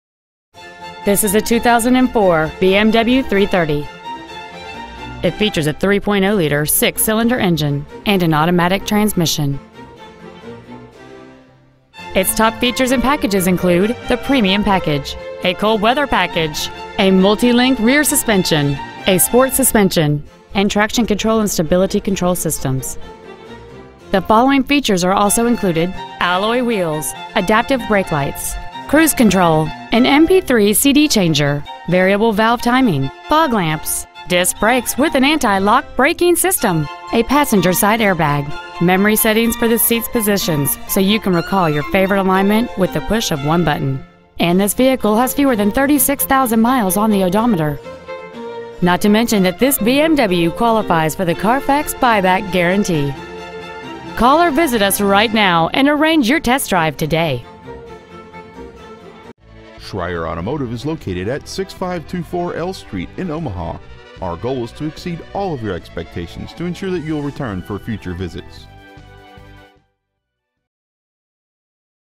Pre-Owned 2004 BMW 330Ci Omaha NE 68117
We have been honored to serve the Omaha NE area , we promise that your experience at our dealership will exceed your expectations !

 Year : 2004
 Make : BMW
 Model : 330Ci
 Engine : 3.0L (183) DOHC 24-valve I6 engine
 Trans . : Automatic
 Exterior : Gray
 Miles : 35,112
 Interior : Black
 Stock : L13375